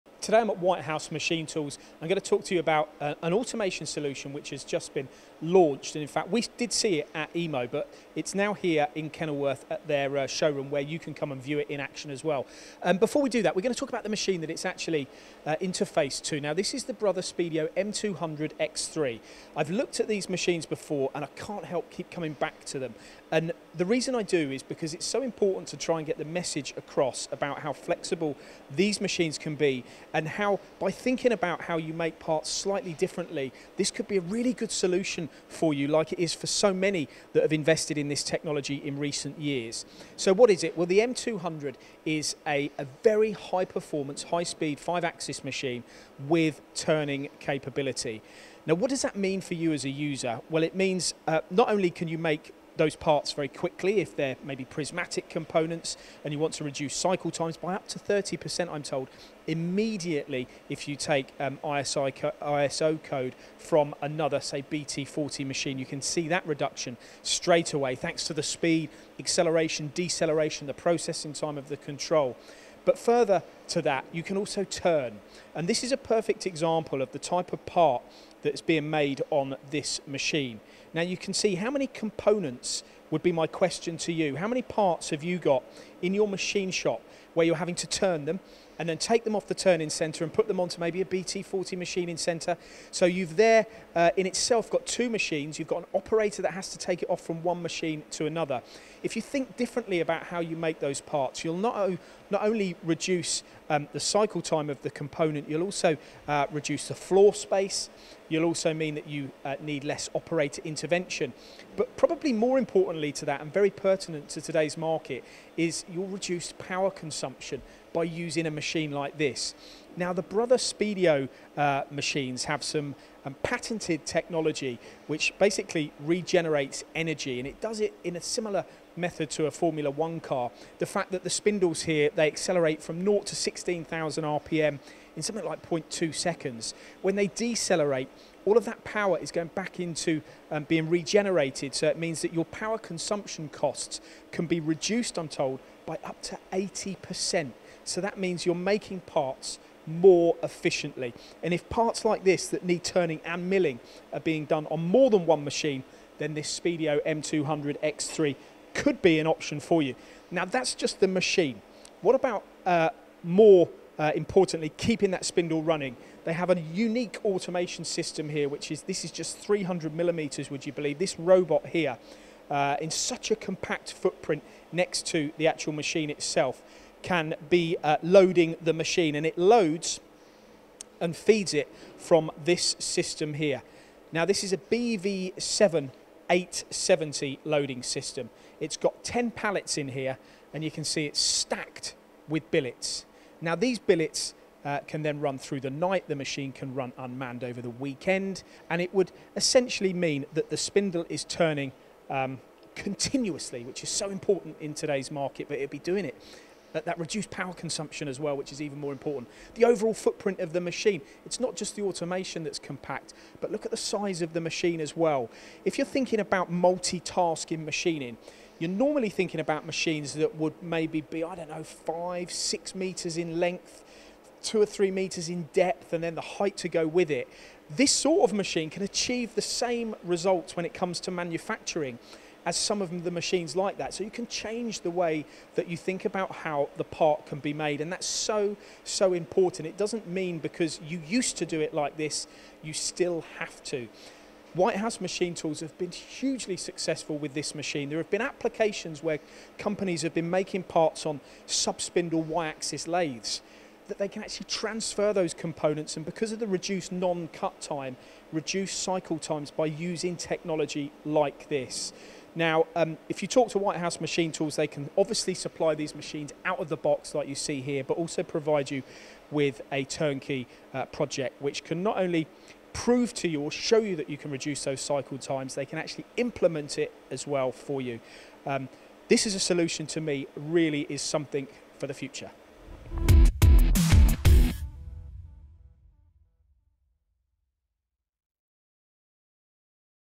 No need for a turning centre if you buy one of these - BROTHER SPEEDIO
The BROTHER SPEEDIO M200 machine offers unrivalled flexibility, superior performance. It's fast and flexible. The key to reducing cycle times is reducing non cut time and operator handling, this SPEEDIO machine does it all